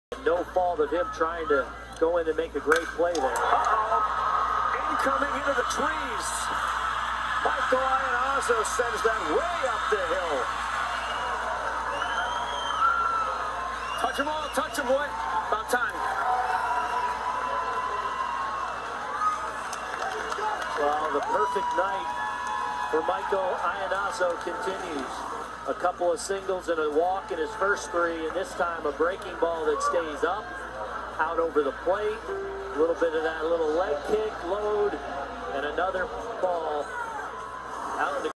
Michael Iannazzo 2 Run Home Run - 2017 LLWS Washington vs Connecticut
Man CT really gave Washington an Ass Whooping